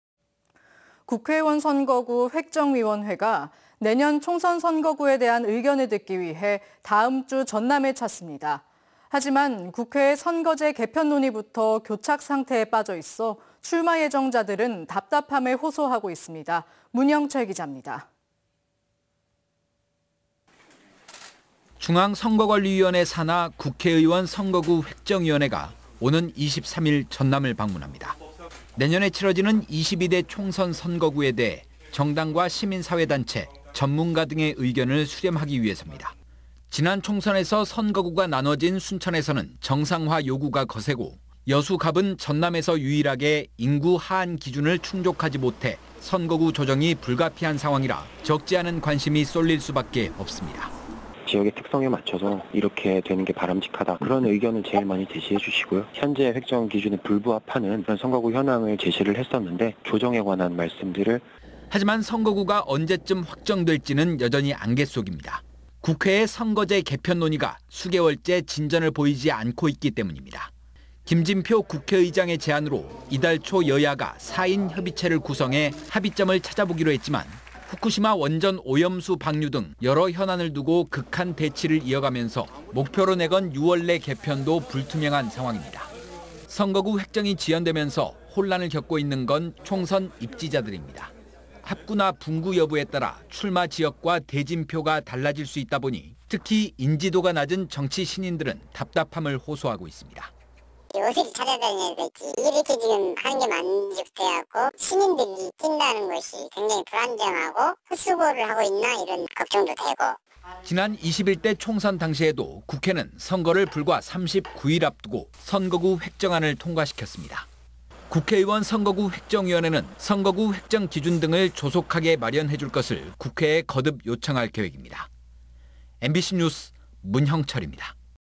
기약 없는 선거구 획정.. 총선 입지자 "혼란" -R (230616금/뉴스데스크)
'국회의원 선거구 획정위원회'가 내년 총선 선거구에 대한 의견을 듣기 위해
다음 주 전남을 찾습니다. 하지만, 국회의 선거제 개편 논의부터 교착상태에 빠져 있어
출마 예정자들은 답답함을 호소하고 있습니다.

문형철 기자입니다.

◀ＶＣＲ▶

중앙선거관리위원회 산하
'국회의원 선거구 획정위원회'가
오는 23일 전남을 방문합니다.

내년에 치러지는 22대 총선 선거구에 대해
정당과 시민사회단체, 전문가 등의 의견을
수렴하기 위해서입니다.

지난 총선에서 선거구가 나눠진 순천의 경우
정상화 요구가 거세고,

여수갑은 전남에서 유일하게
인구 하한 기준을 충족하지 못해
선거구 조정이 불가피한 상황이라
적지 않은 관심이 쏠릴 수 밖에 없습니다.

◀ＩＮＴ▶
"지역의 특성에 맞춰서 이렇게 (조정)되는 게
바람직하다는 의견을 제일 많이 주시고요.
현재 획정 기준에 부합하지 않은
선거구 현황을 제시했는데 조정에 관한 말씀들을..."

하지만, 선거구가 언제쯤 확정될지는
여전히 안갯속입니다.

국회의 선거제 개편 논의가
수개월째 진전을 보이지 않고 있기 때문입니다.

김진표 국회의장의 제안으로
이달 초 여˙야가 '4인 협의체'를 구성해
합의점을 찾아보기로 했지만,

후쿠시마 원전 오염수 방류 등 여러 현안을 두고
극한 대치를 이어가면서
목표로 내건 6월 내 개편도 불투명한 상황입니다.

선거구 획정이 지연되면서
혼란을 겪고 있는 건 총선 입지자들입니다.

합구나 분구 여부에 따라
출마 지역과 대진표가 달라질 수 있다 보니
특히, 인지도가 낮은 정치 신인들은
답답함을 호소하고 있습니다.

◀ＳＹＮ▶
"어디를 찾아다녀야 될지, 이렇게 하는 게 맞는지부터 해서
신인들이 뛴다는 것이 굉장히 불안정하고
헛수고를 하고 있나, 이런 걱정도 되고..."

지난 21대 총선 당시에도
국회는 선거를 불과 39일 앞두고
선거구 획정안을 통과시켰습니다.

국회의원선거구 획정위원회는
선거구 획정 기준 등을 조속하게 마련해 줄 것을
국회에 거듭 요청할 계획입니다.

MBC NEWS 문형철입니다. ◀ＥＮＤ▶